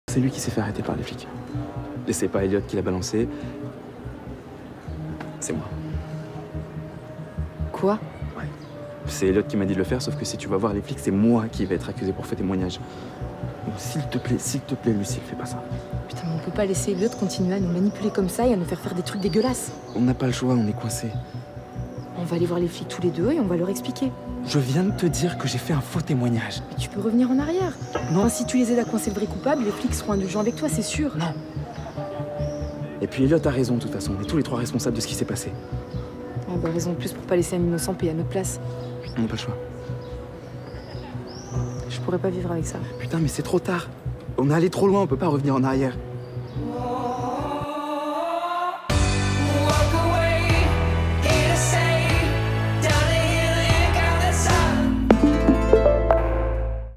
Charles a fait un faux témoignage pour couvrir Eliott (Ep37)
Extrait de l'épisode 37 du mardi 16 octobre 2018.